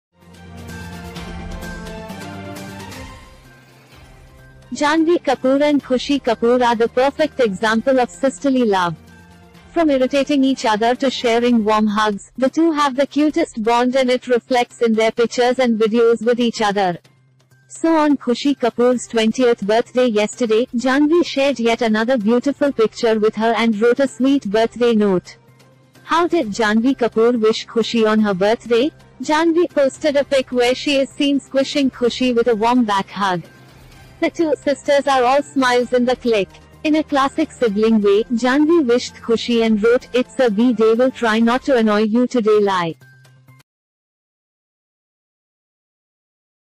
Janhvi Kapoor tried not to annoy Khushi because on her birthday. No promises, though
In a first, Travancore temple board to appoint ST priest

0:10 Janhvi Kapoor tried not to annoy Khushi because on her birthday. No promises, though